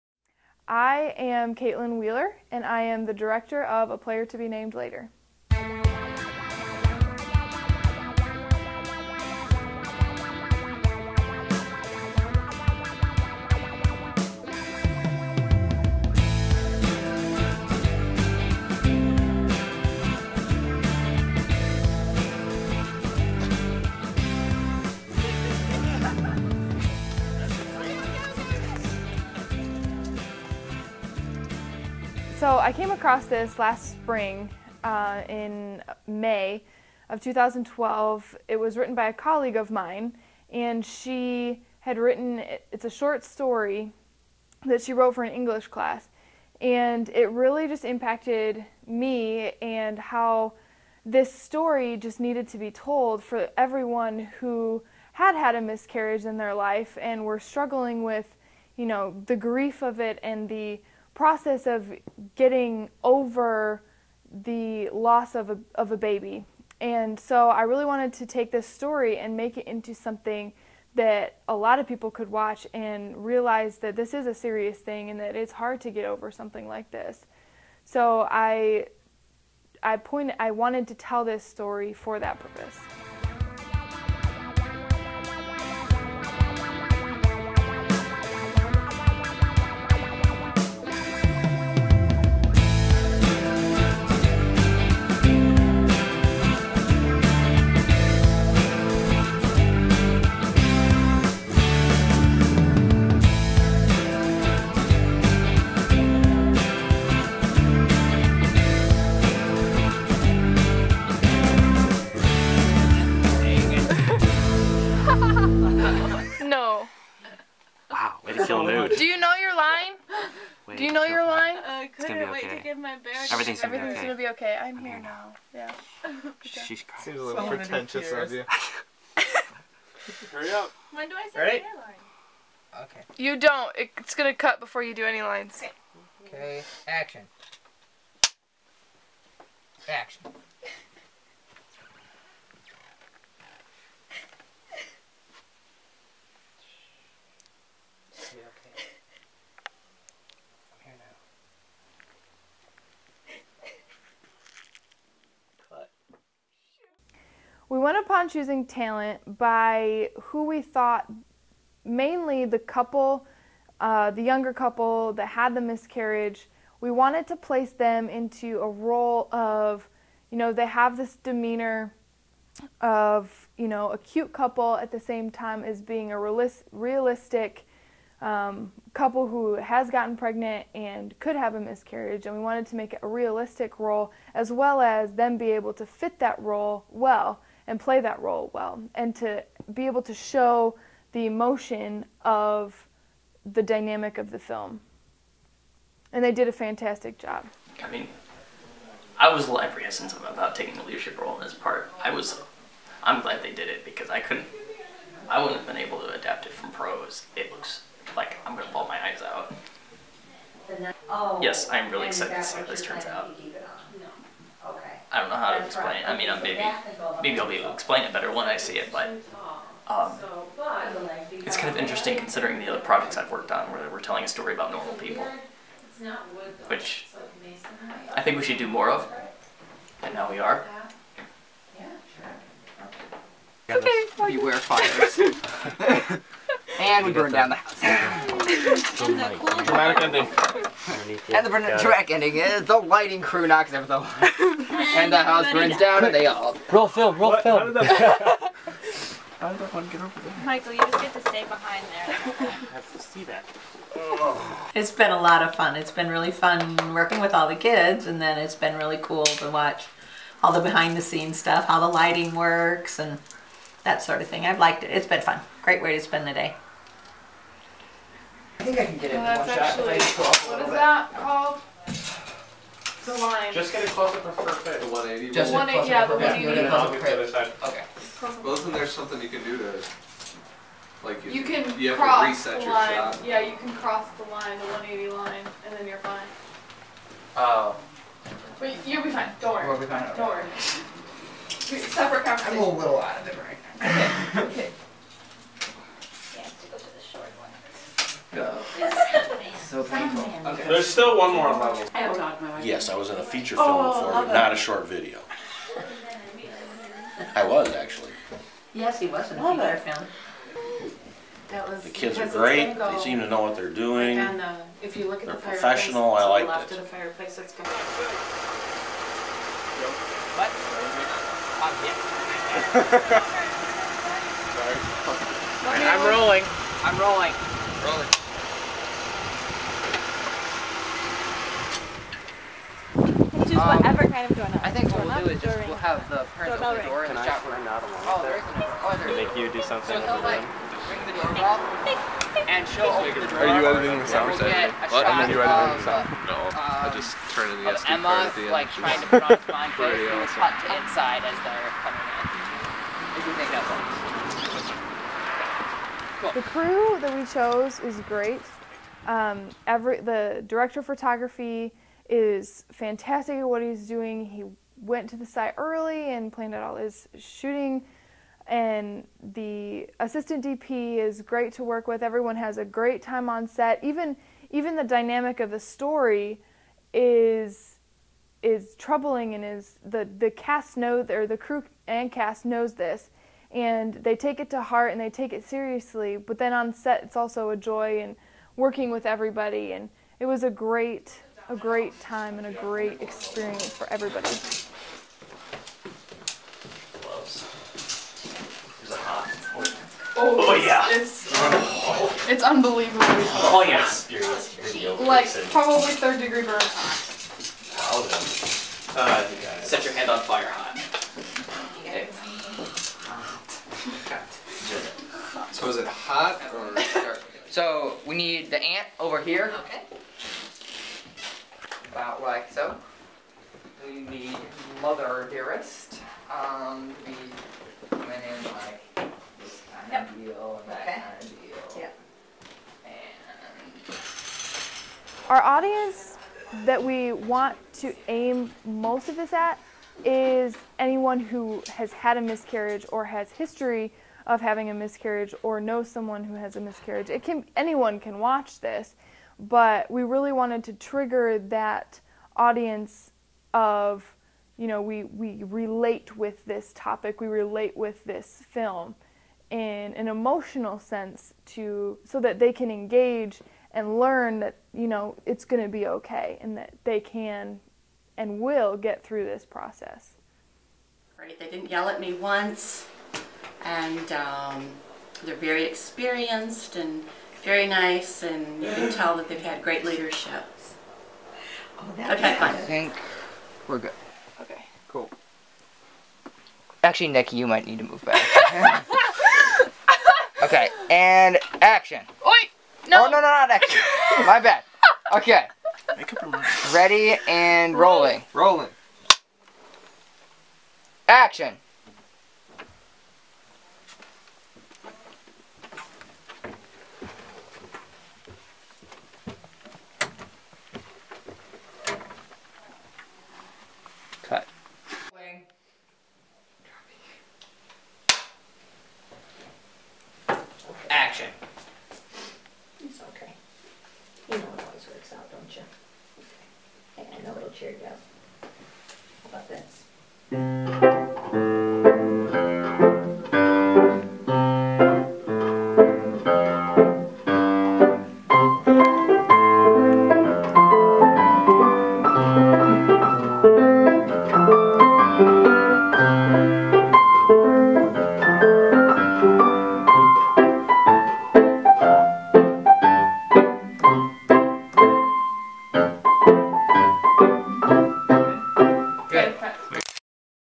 Behind the scenes of "A player to Be Named Later"
This is the Behind the Scenes Footage of "A Player to Be Name Later". Director commentary and cast interviews.